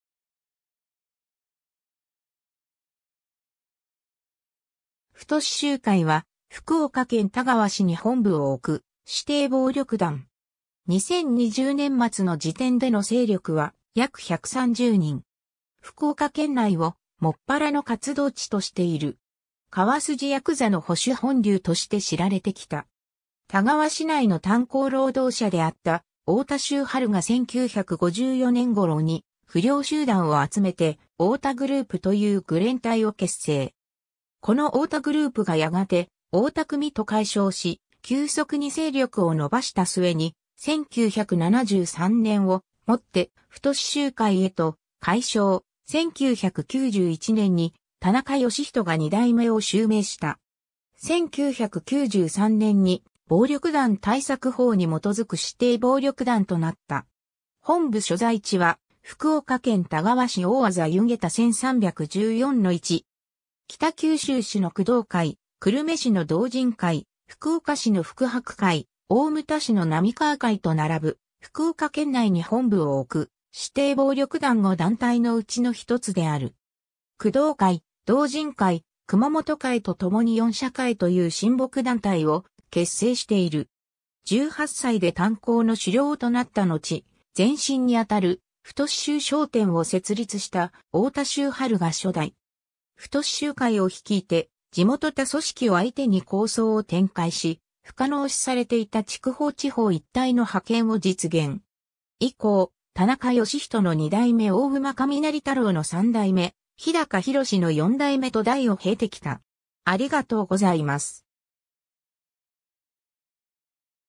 太州会
太州会（たいしゅうかい）は、福岡県田川市に本部を置く指定暴力団。
2020年末の時点での勢力は約130人(構成員:約80人、準構成員等:約50人)。
福岡県内を専らの活動地としている。
“川筋ヤクザの保守本流”として知られてきた。
 田川市内の炭鉱労働者であった太田州春が1954年（昭和29年）頃に不良集団を集めて「太田グループ」という愚連隊を結成。
この太田グループがやがて“太田組”と改称し、急速に勢力を伸ばした末に、1973年（昭和48年）をもって“太州会”へと改称、1991年（平成3年）に田中義人が二代目を襲名した。
1993年（平成5年）に暴力団対策法に基づく指定暴力団となった。
 本部所在地は福岡県田川市大字弓削田1314-1。
北九州市の工藤会、久留米市の道仁会、福岡市の福博会、大牟田市の浪川会と並ぶ、福岡県内に本部を置く指定暴力団5団体のうちの一つである。
工藤会、道仁会、熊本會とともに四社会という親睦団体を結成している。
 18歳で炭鉱の首領となったのち、前身にあたる「太州商店」を設立した太田州春が初代。
太州会を率いて地元他組織を相手に抗争を展開し、不可能視されていた筑豊地方一帯の覇権を実現。
以降、田中義人の二代目〜大馬雷太郎の三代目〜日高博の四代目〜と代を経てきた。